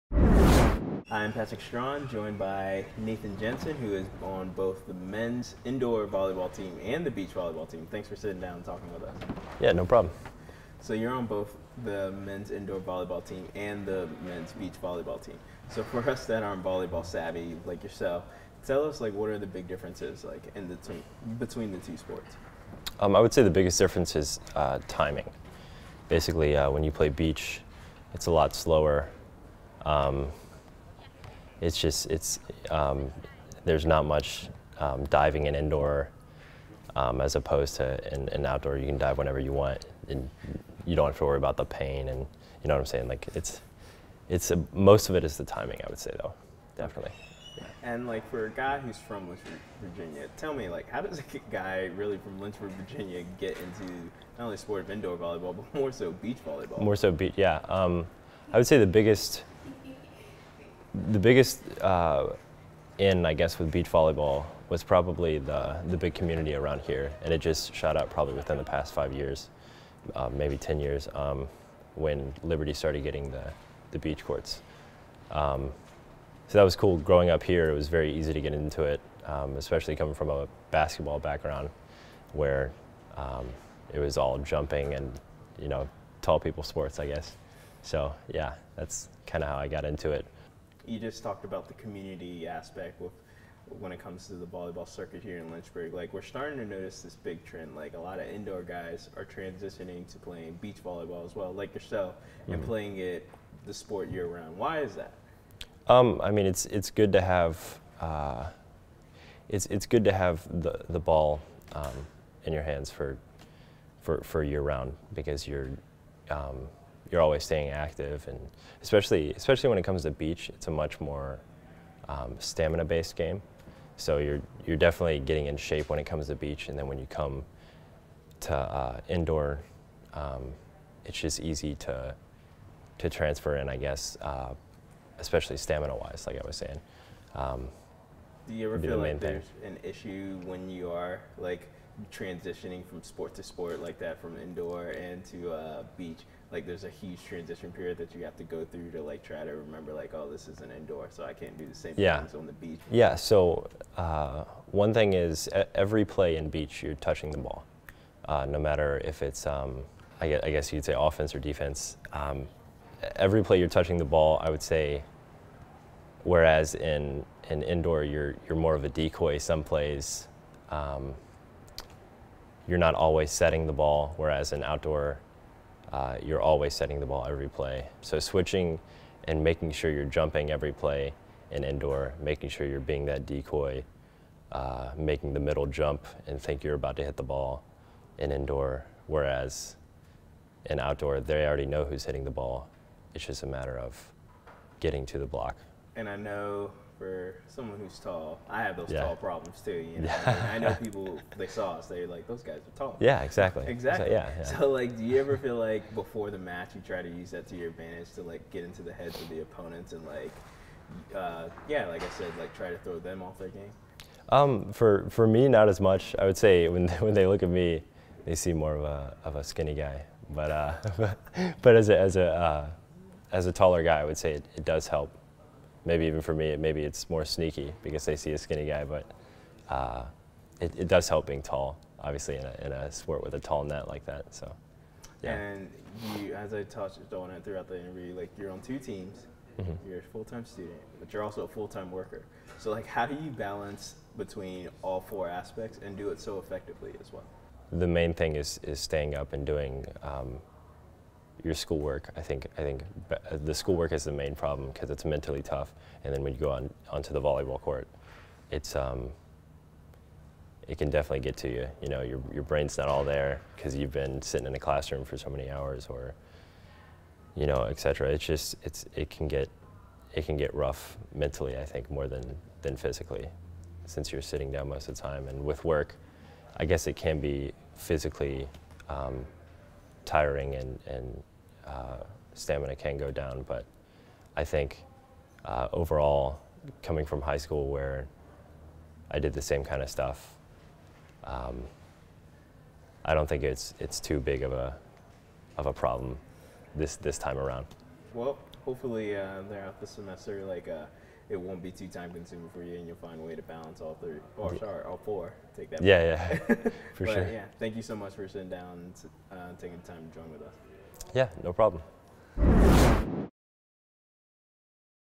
Face to Face with Nathan Jensen
Liberty Club Sports Patrick Strawn sits down with Liberty Mens Indoor and Beach Volleyball athlete Nathan Jensen